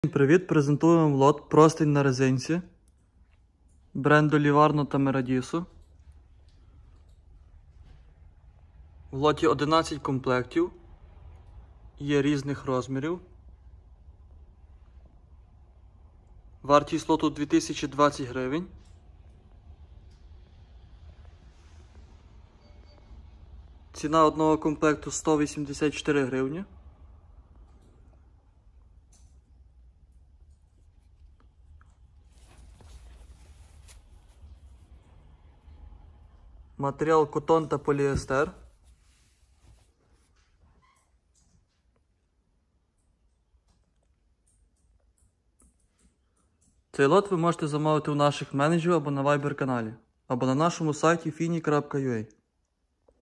Лот M05 32826, Простинь на резинці Livarno Home, вага 7 кг (11 шт), Ціна 2 020 грн
Простинь на резинці Livarno Home + Meradiso
140-160*200 - 2 шт
90-100*200 - 7 шт
180-200*200 - 2 шт